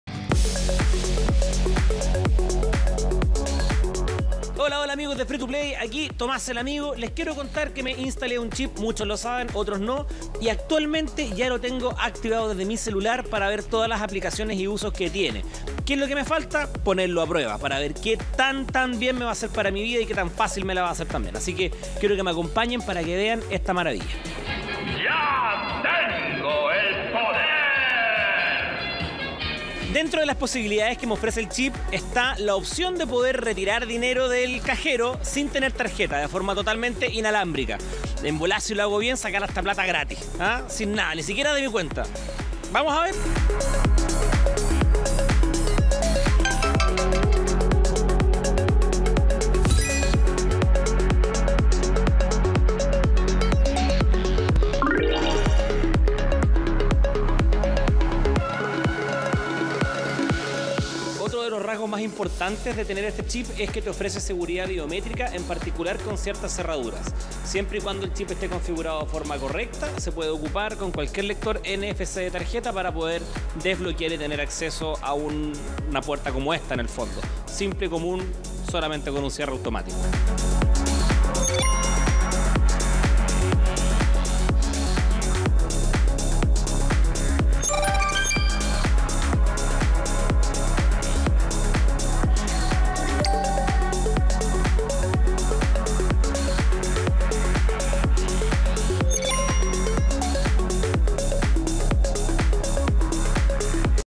La modificación humana a través de un CHIP
Tomás tiene un CHIP en su cuerpo.